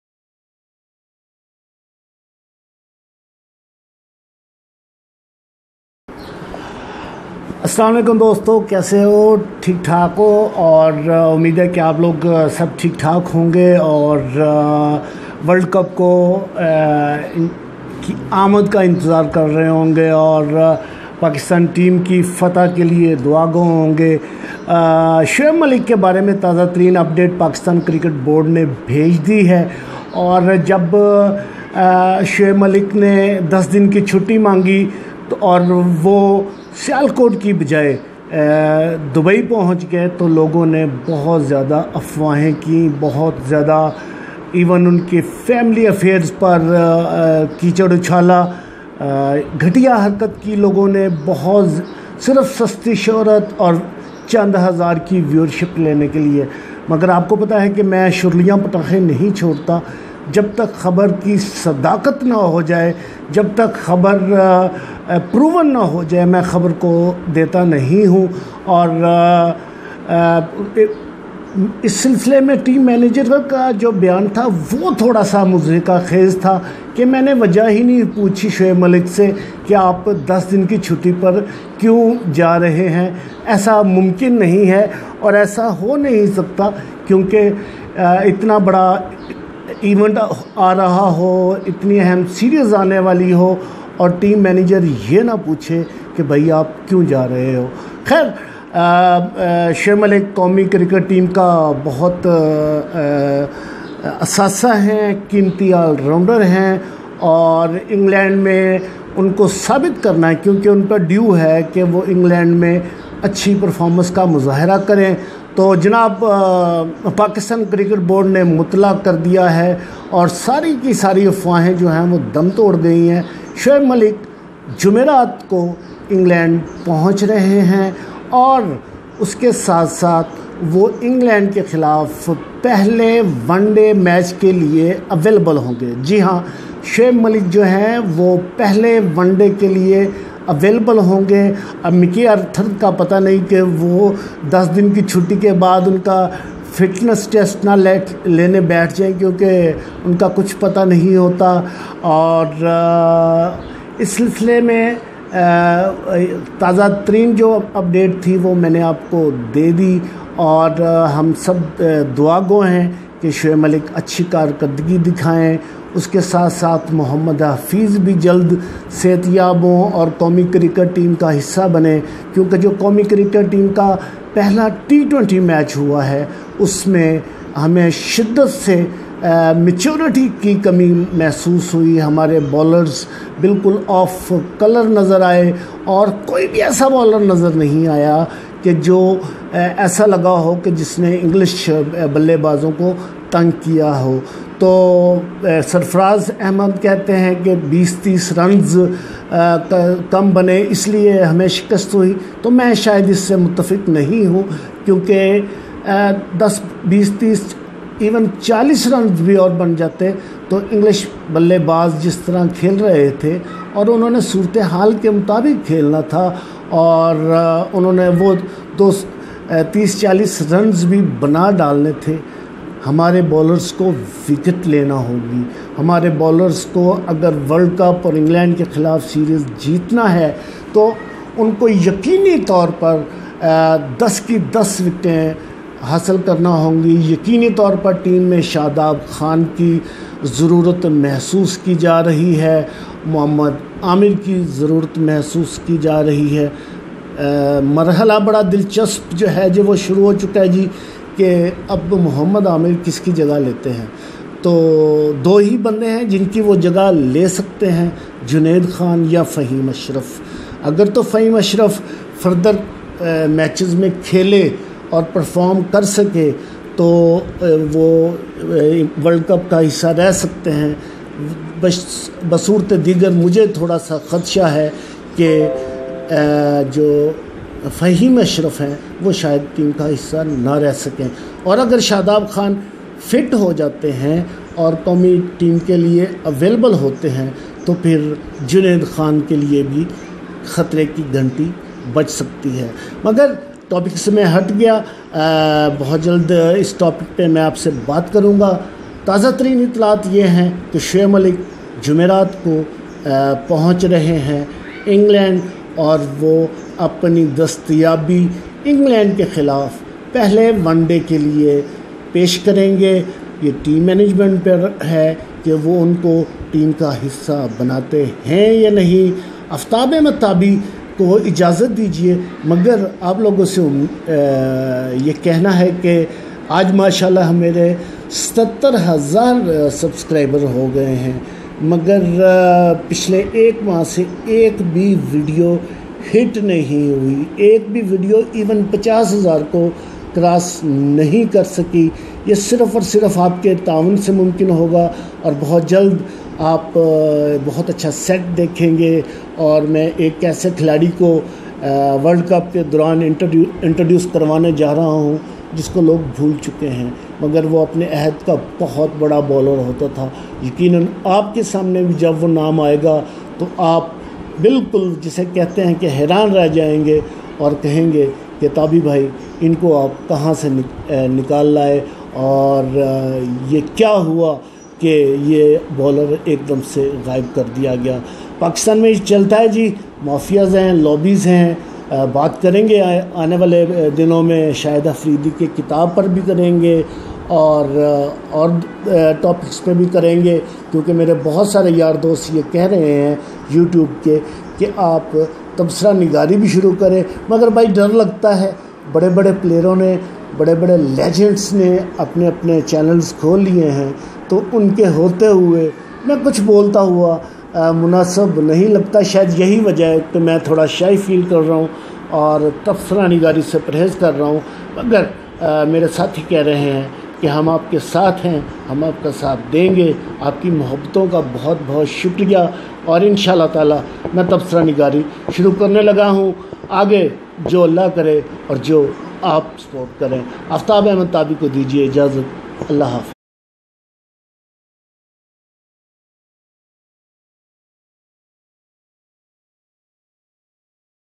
Most Latest Up date About Shoaib Malik, Read Correction in description
Sorry Guys its my Mistake , Shoaib Malik will be available for 2nd One Day against England, Slip of Tongue hahahaha